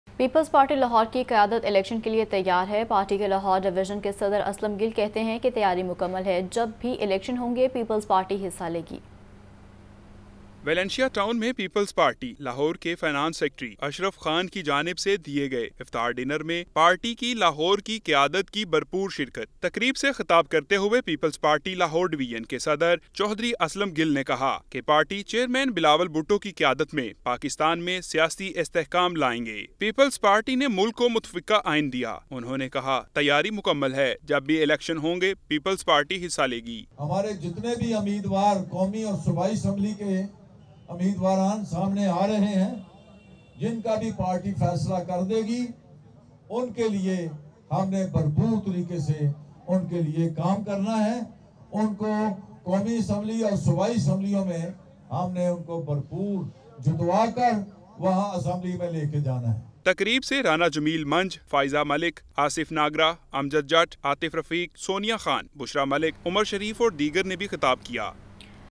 Peoples Party Election Kay Liye Tayar Hai, PPP Rehnuma | Lahore News HD
Pakistan Peoples Party is ready for elections